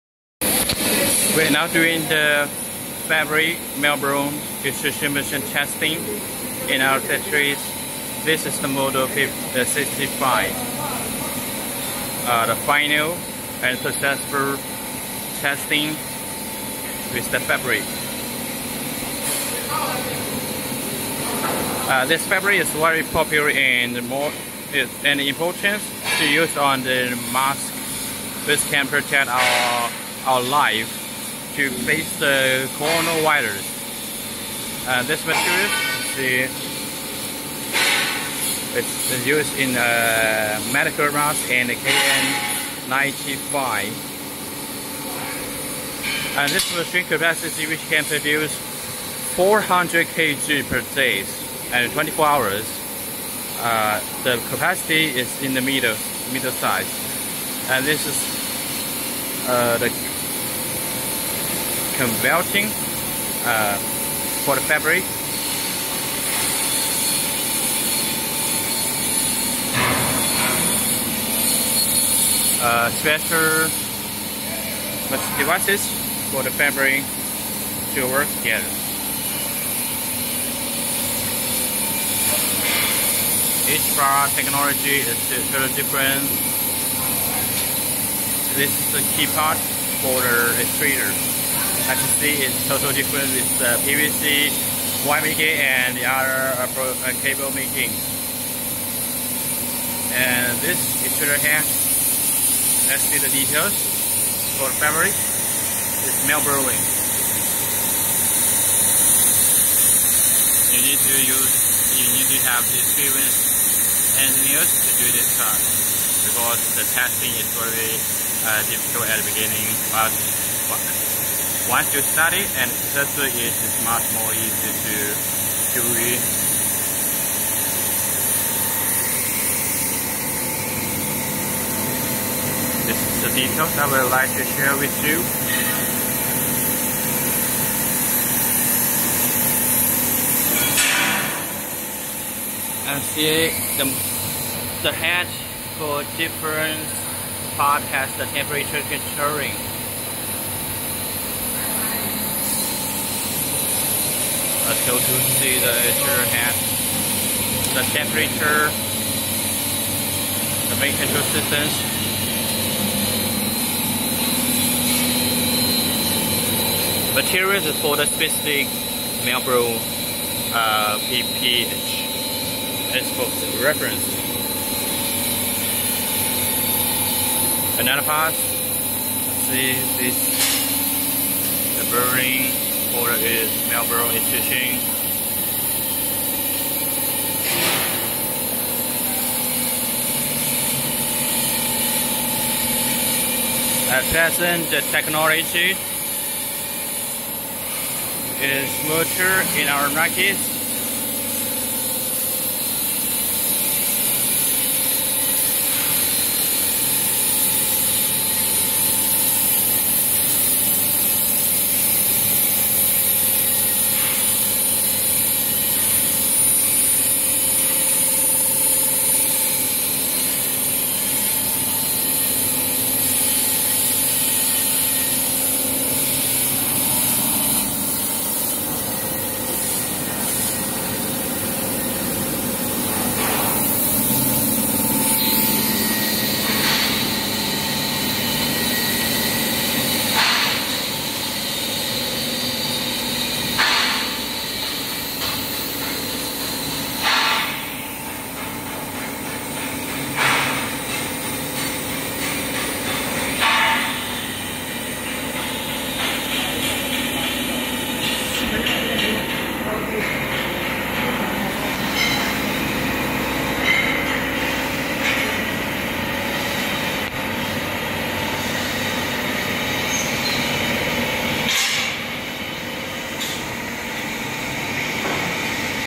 How to make mask? melt blown fabric, nonwoven fabric extruder
This is the trial run of our meltblown fabric and woven extruder. It is a 65 type, 400 KG / 24 hour capacity, 85 KW, 380 V model

Some of our customers bought masks from their market and brought them more benefits.

Hope the video will help you, and the HOOHA team looks forward to doing business with you to seize the opportunity.